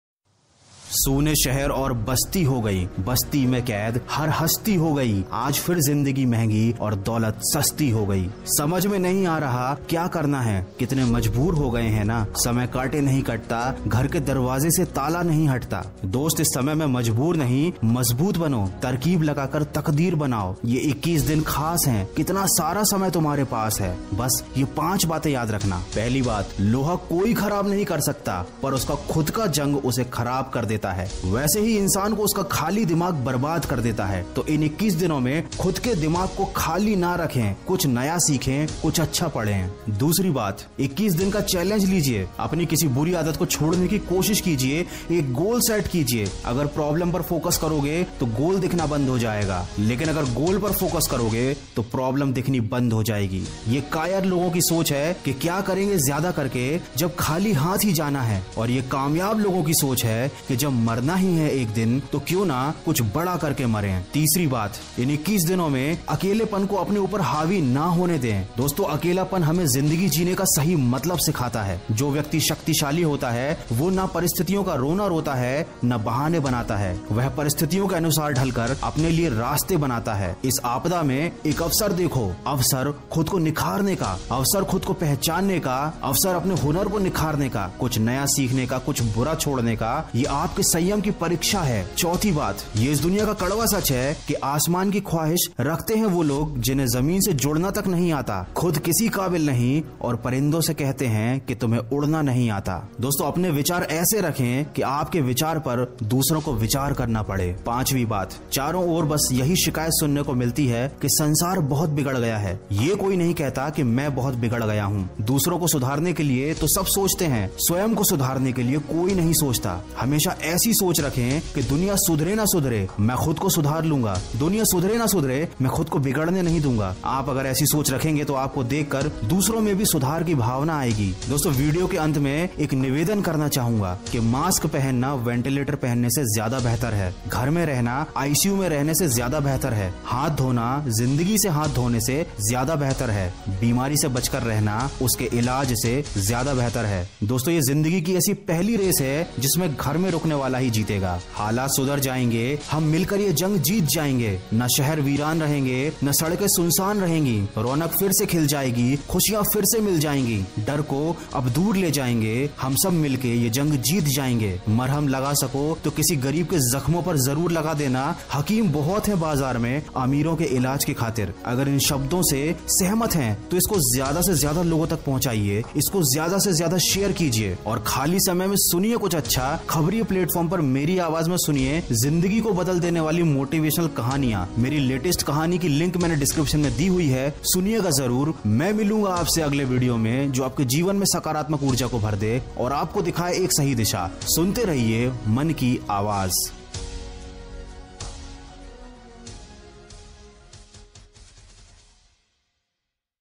21day lokdown I Best powerfull Motivational video in Hind inspirational speech- New Life
21 days lockdown to change your Life
सुनिए मेरी आवाज़ में मोटिवेशनल कहानियां